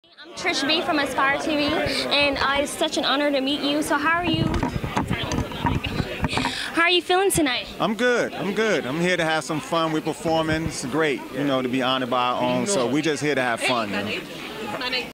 DJ Kay Gee Naughty By Nature Red Carpet @ Black Music Honors DreamSpireTV
DJ Kay Gee Naughty By Nature Red Carpet @ Black Music Honors
Trish B A-Spire Magazine. DreamSpireTV videographer Greg Giles